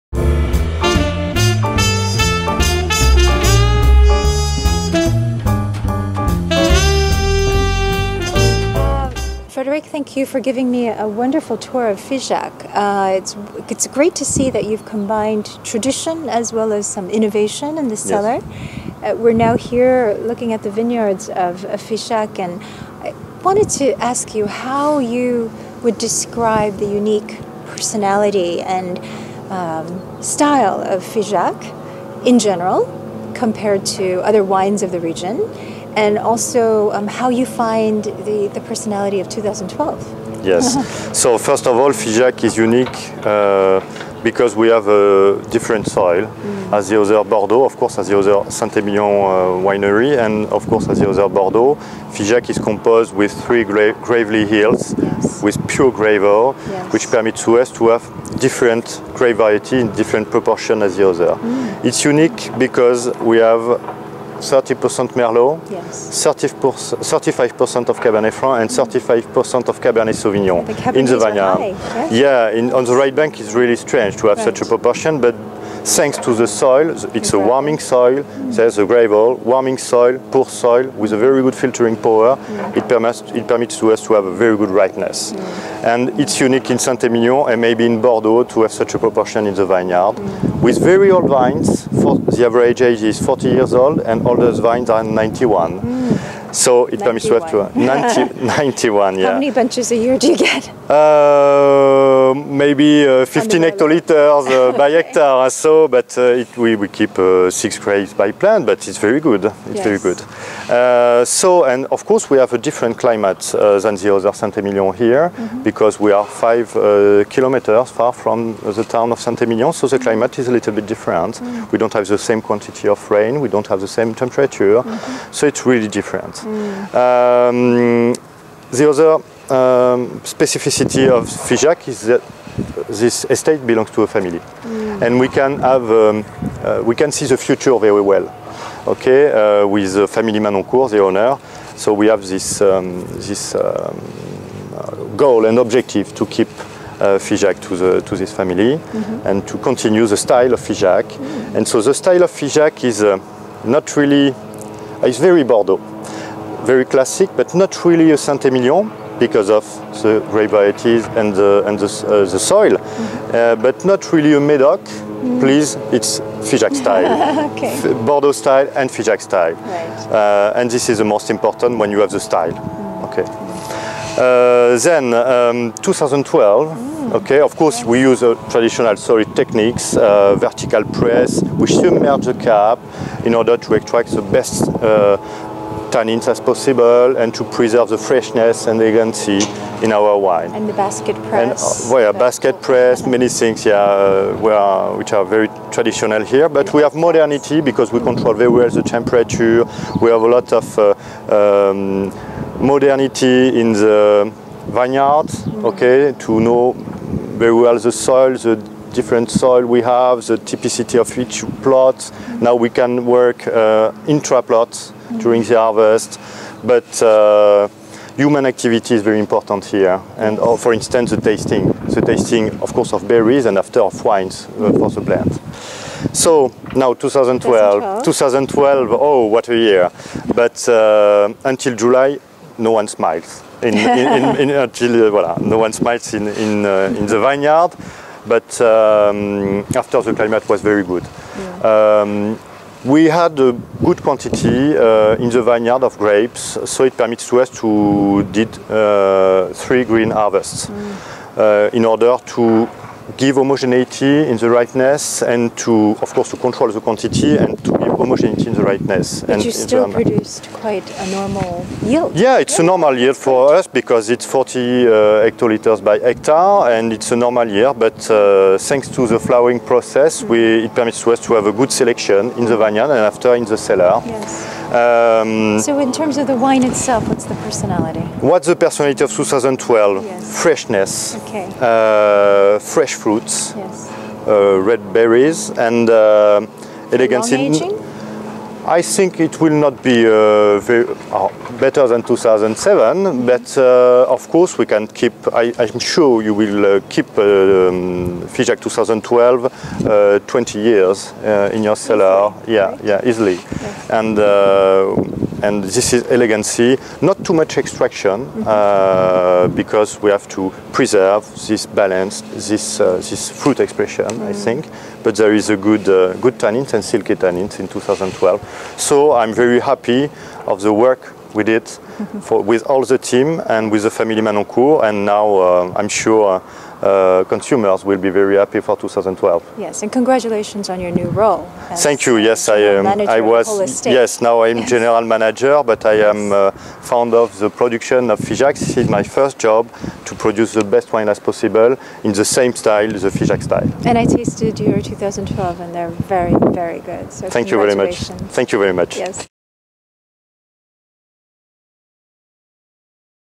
Jeannie Cho Lee MW Interviews Frederic Faye from Chateau Figeac
Frederic Faye talks about the uniqueness and the vintage of 2012 in Chateau Figeac.